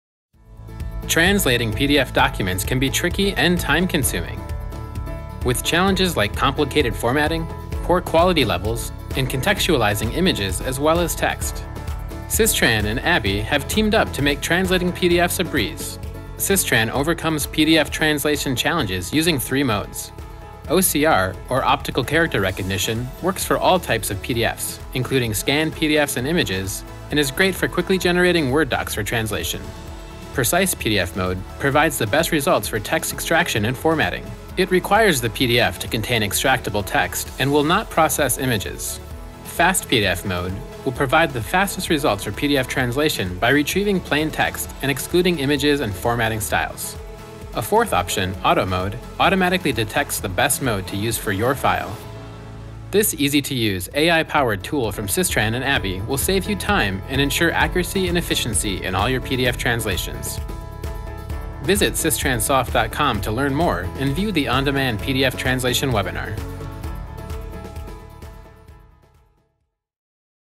Overcoming PDF Translations Challenges
Get a better understanding of the challenges machine translation engines have when confronted with low quality PDFs.